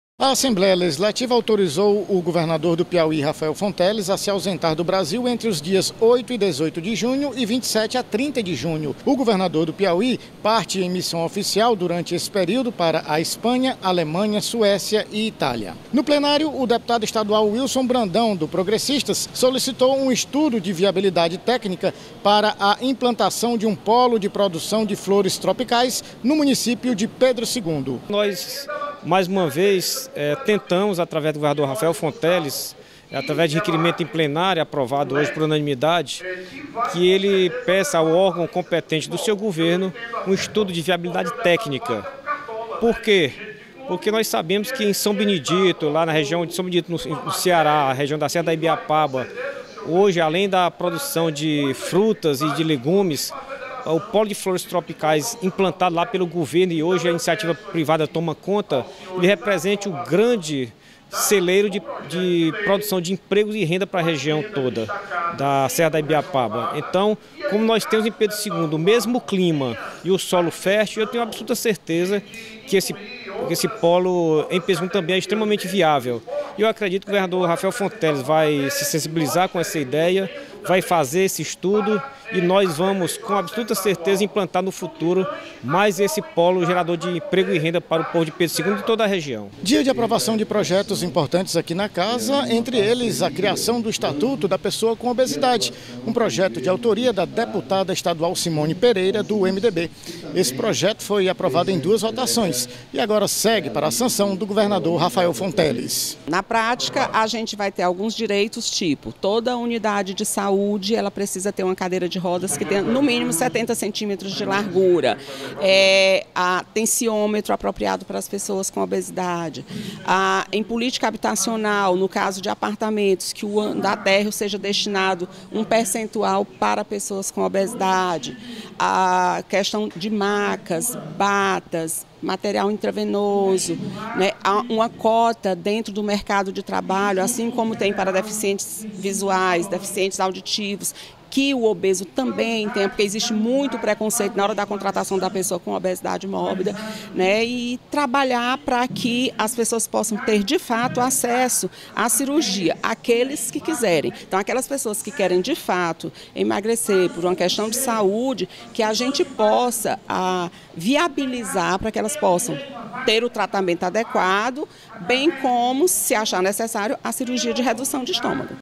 Sessão Plenária 30.05.23 - Aprovado Estatuto da Pessoa com Obesidade
Durante a sessão plenária desta terça-feira (30), foi aprovado o Estatuto da Pessoa com Obesidade. Os deputados aprovaram também um estudo de viabilidade para a implantação de um polo de produção de flores tropicais na cidade de Pedro II. Mais informações na reportagem de Ricardo Moura Fé e Luís Carlos Paiva, da TV Assembleia.